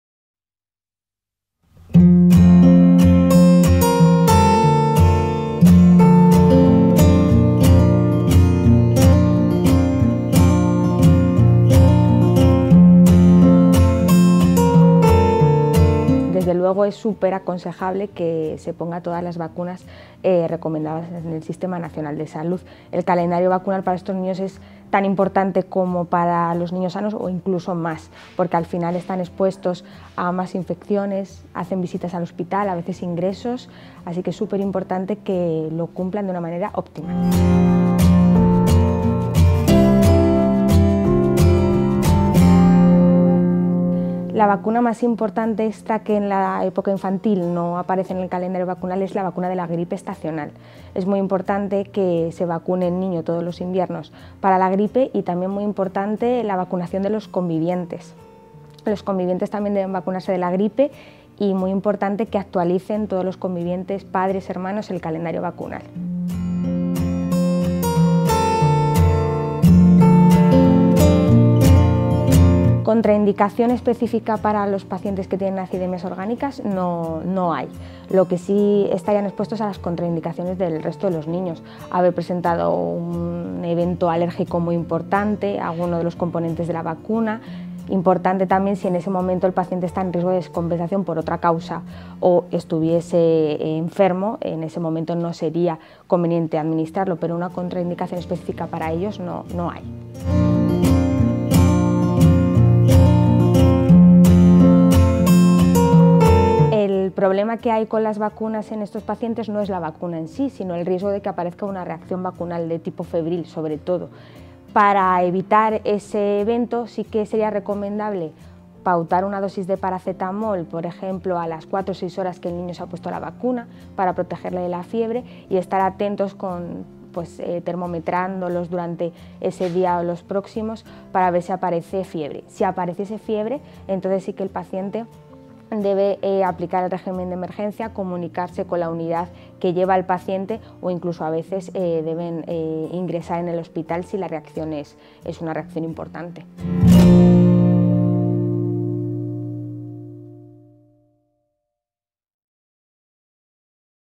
SILVIA CHUMILLAS CALZADA
Entrevista a la Dra Silvia Chumillas, de la Unidad de enfermedades metabólicas y mitocondriales del CSUR 12 de Octubre de Madrid, sobre la vacunación en acidemias orgánicas.